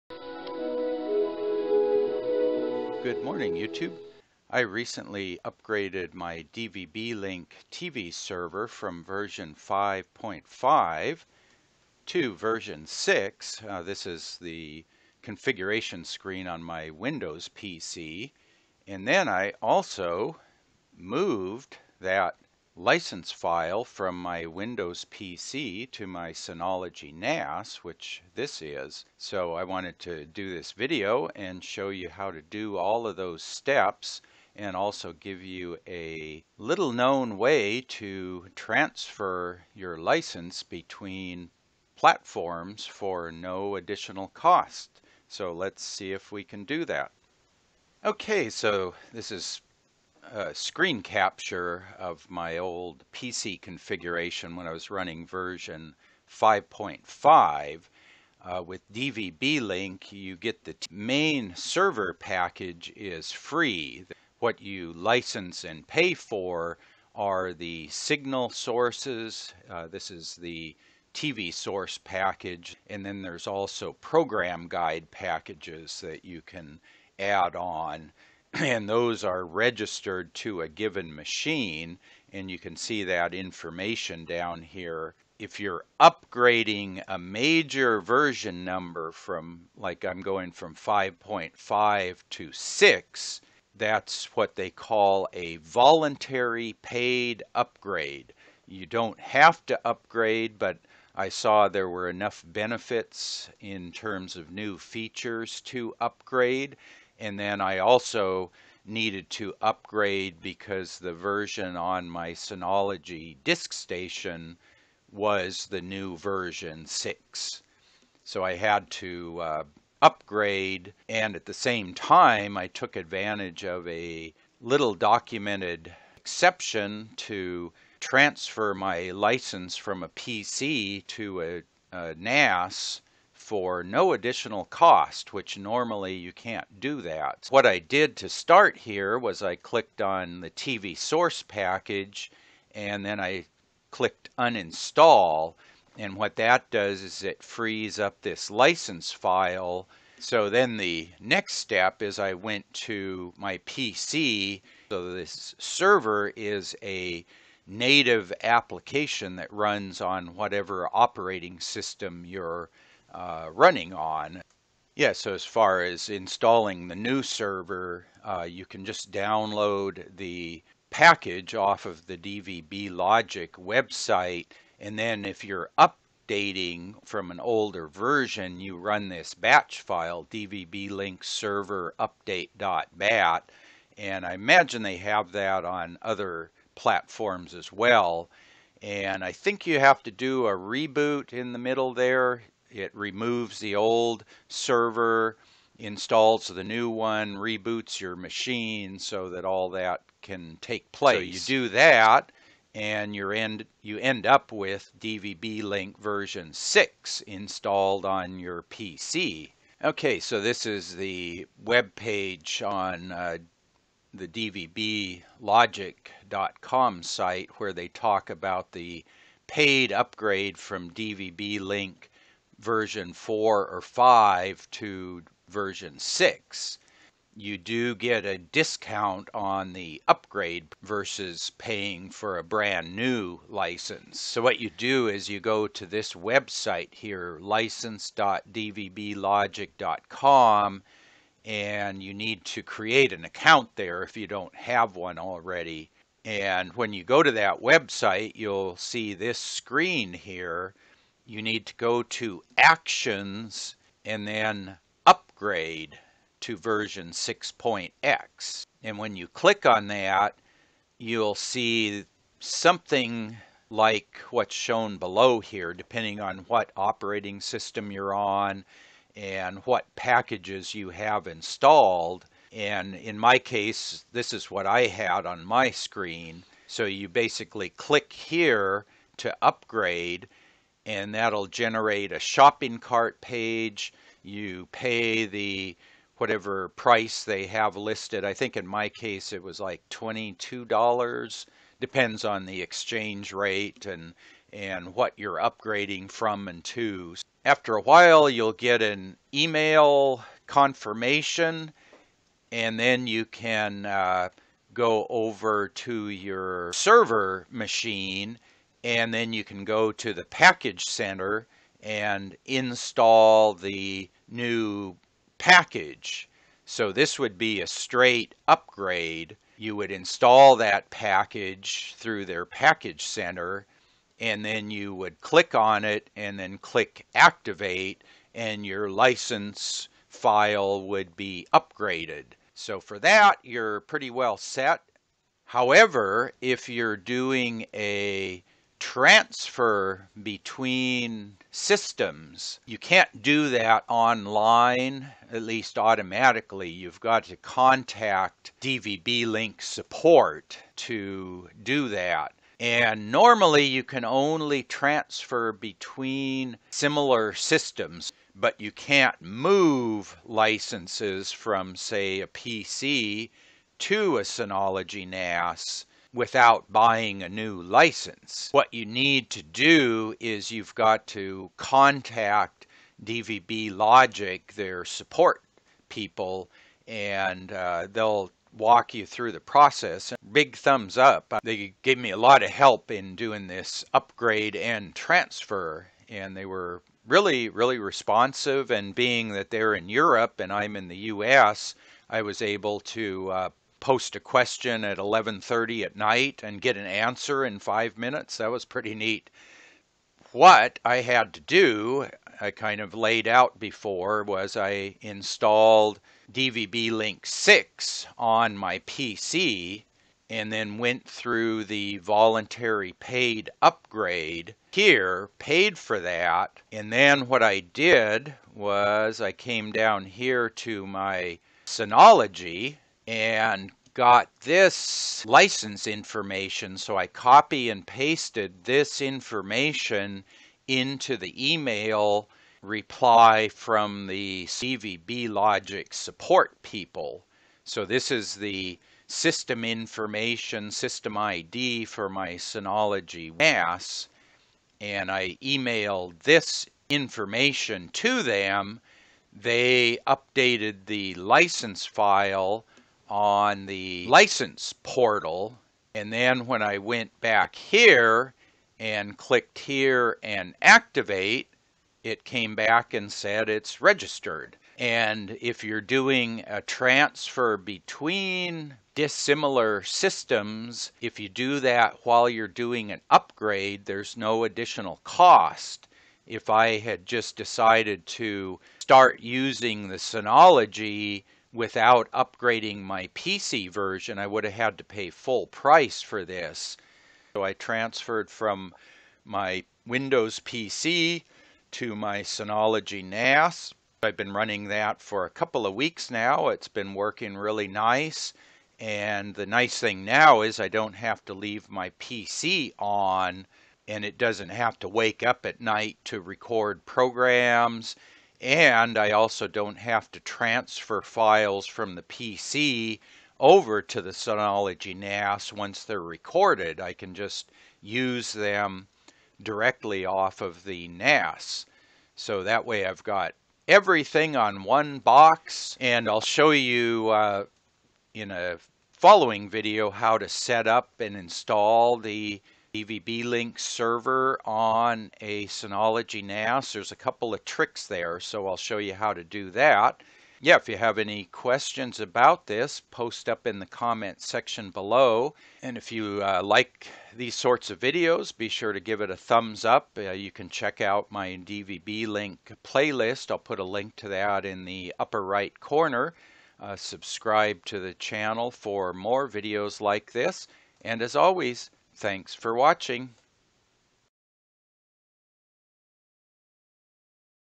DvbLink TV Server version 6, Upgrade and Transfer
Walking through the DvbLink TV server upgrade and license transfer process.

I've been running DvbLink TV server since v4 about 4 yrs. ago.  Over that time, I upgraded to v5 and also transferred that license from one PC to another PC.

Upgrades can be done without opening a support ticket.  You uninstall the old licensed packages on thew DvbLink Products screen.  Then visit the License Portal and click on Actions then Upgrade to version X and follow the instructions.  If required, a payment screen will take you through those steps.  Once you receive notification of the upgrade, you can install and activate the new version packages on your server.

Transfers between similar systems are free, but you generally can't transfer from one type of system, say a PC, to another type, like a Synology NAS.  For a transfer, you'll uninstall and licensed packages on the old server then install them on the new server.  Finally send the new server ID information to DvbLogic support along with an explanation that you want to transfer from PC-1 to PC-2, etc.

You can't normally transfer a license between machines of different architecture, like a PC to a NAS.  But if you combine that transfer with a paid upgrade, you can do both for the discounted upgrade price.

In my case a new license would have cost around US$60, but upgrading and transferring that license from my WIndows PC to my Synology NAS cost about US$22.  If I add the $45 cost of the additional HDHomeRun network tuner, my total cost for the Synology setup was $67.  So the network TV tuner cost me an extra $7.

Some setup tips and tricks for installing the DvbLink server on a Synology NAS: